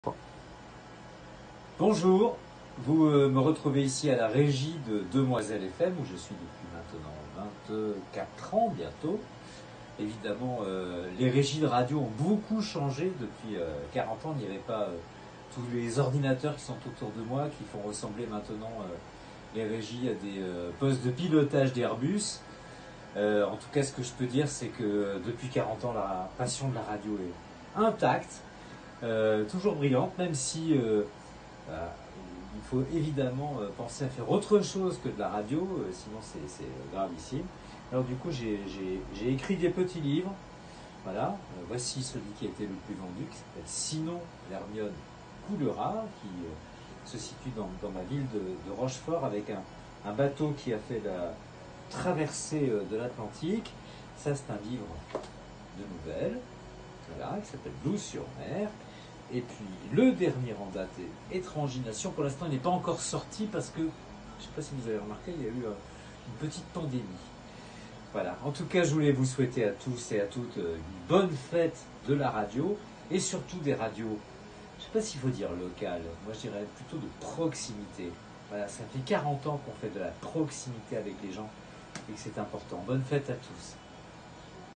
Fête de la radio : des Alpes à la Rochelle et même jusqu’à la littérature ! Arnaud Develde
C'est donc cette semaine la fête de la radio et le 40è anniversaire "des Radios libres". D'où l'opération spéciale mise en place par Jean-Marc Passeron et D!CI Radio 90.2 avec l'aide d'Annie Borgia et de Jean-Luc Bernard mais aussi de Manon et de Mélanie que l'on remercie. Parmi ceux qui n'ont pu se joindre au rassemblement de ce lundi à Chorges et qui ont fait cette épopée, Arnaud De Velde qui a beaucoup aidé au lancement d'Alpes 1 dont il fut le premier et brillant animateur. Il est aujourd'hui à Rochefort et plus que jamais dans la radio mais aussi la littérature…